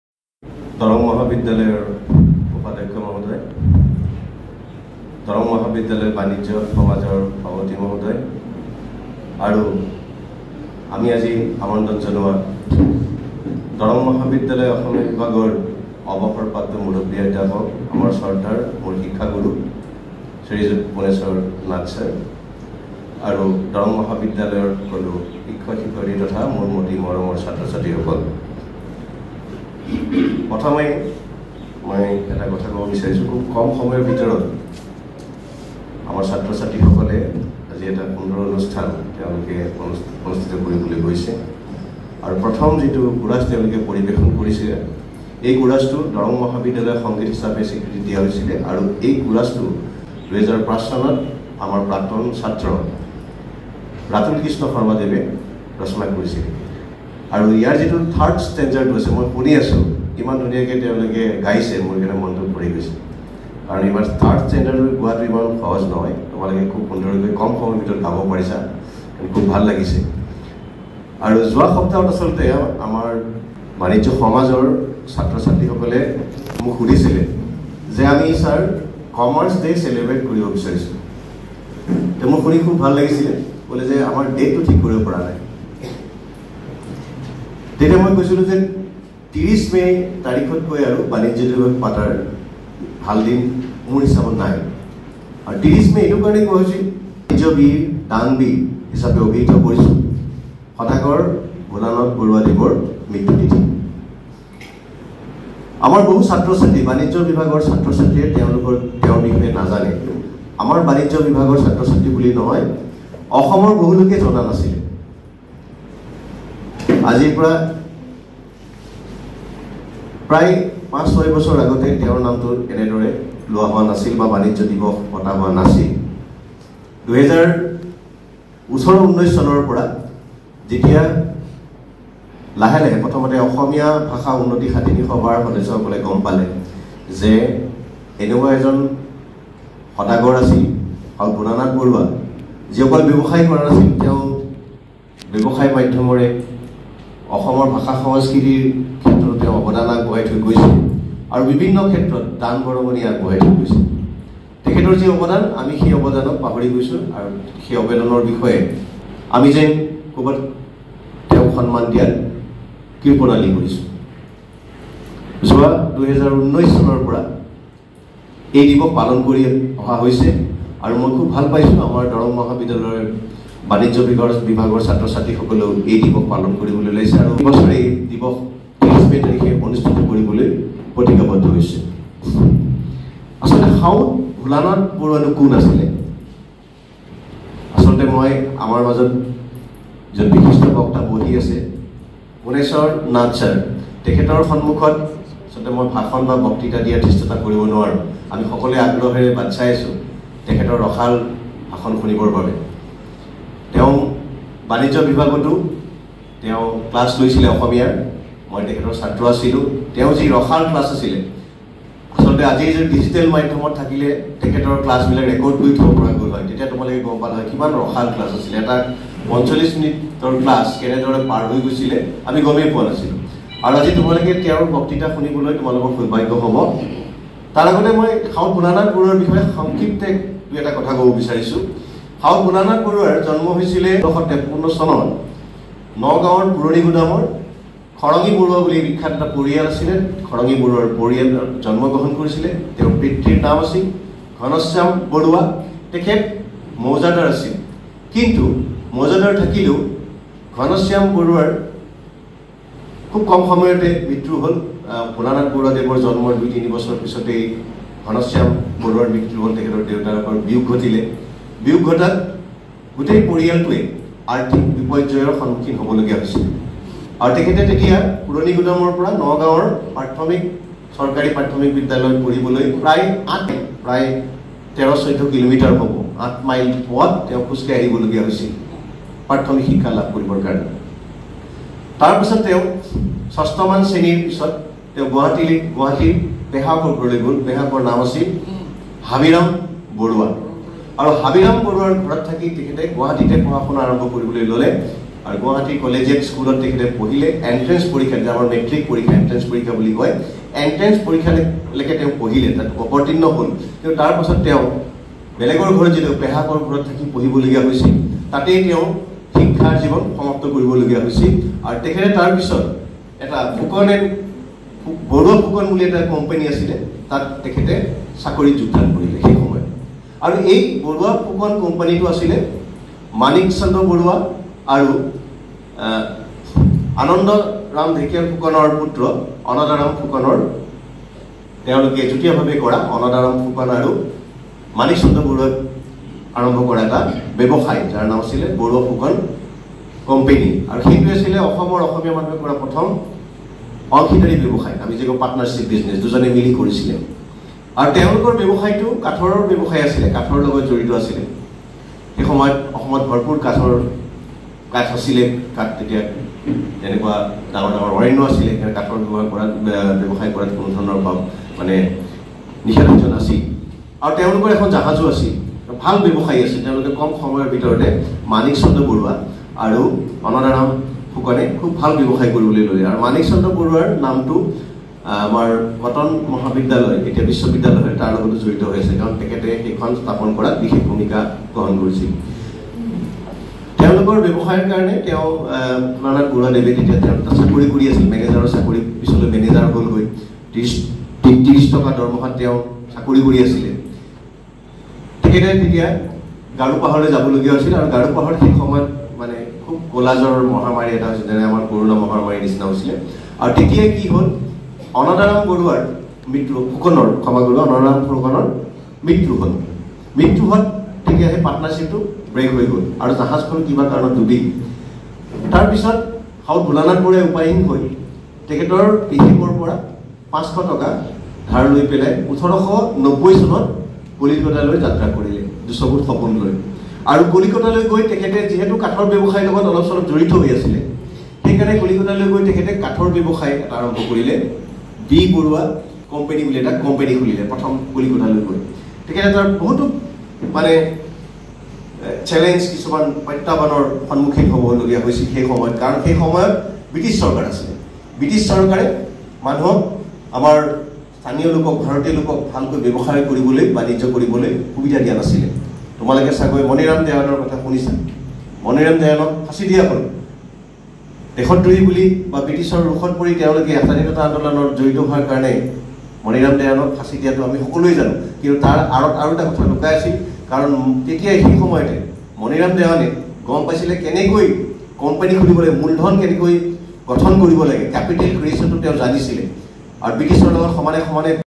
Speech on Sadagor Bholanath Baruah
by Dr. Manoj Kumar Hazarika, Department of Commerce, Darrang College, Tezpur